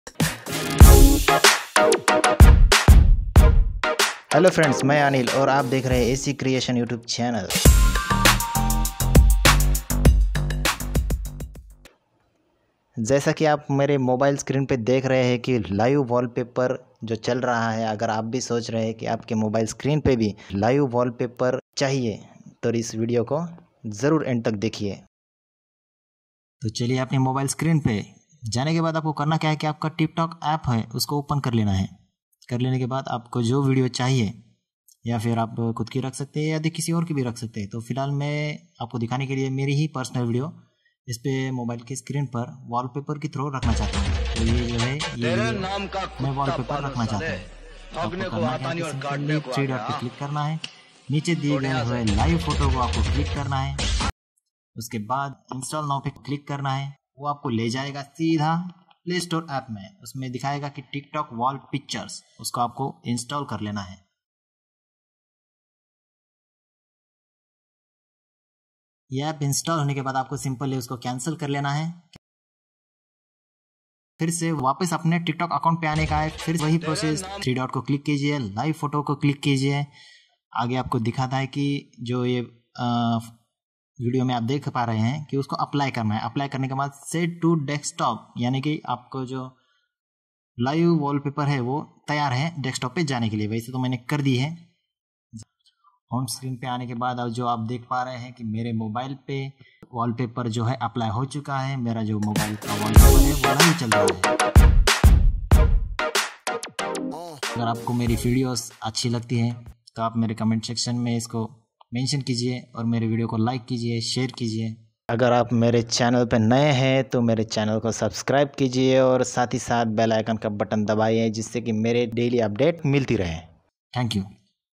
MobileकीHome screenपरTik Tokकि व्हिडिओ कैसे सेट करे/How to set Tik Tok video on Mobile Home screen🕺
MobileकीHome screen परTik Tokकि व्हिडिओ कैसे सेट करे/How to set Tik Tok video on Mobile Home screen🕺

👉green screen mobile portrait video

👉how to set live wallpaper on your mobile screen